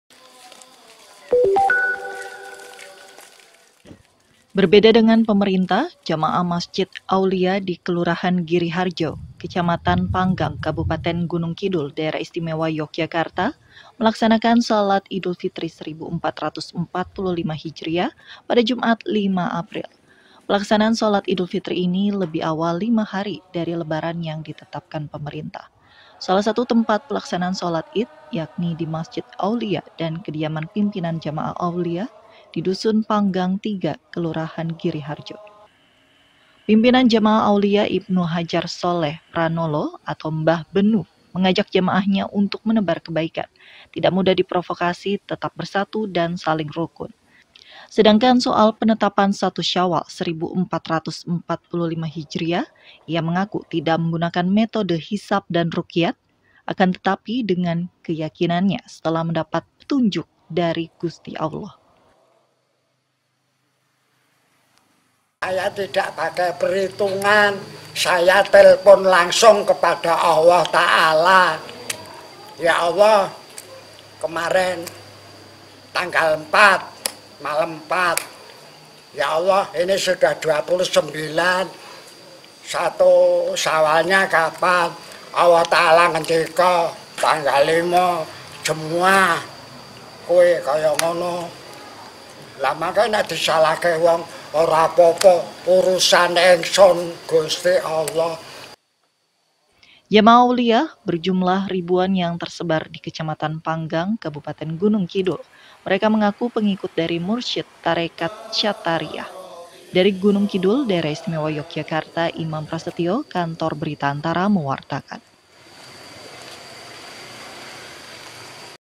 Lebih awal, jamaah Aolia gelar shalat Idul Fitri 1445 H hari ini
ANTARA - Ribuan jamaah Masjid Aolia yang tersebar di Kecamatan Panggang, Kabupaten Gunungkidul, D.I. Yogyakarta merayakan Idul Fitri 1445 Hijriah lebih awal, pada Jum’at (5/4). Mereka mendatangi masjid-masjid Aolia di sejumlah titik untuk melaksanakan ibadah shalat Idul Fitri.
(Imam Prasetyo Nugroho/Soni Namura/Ludmila Yusufin Diah Nastiti)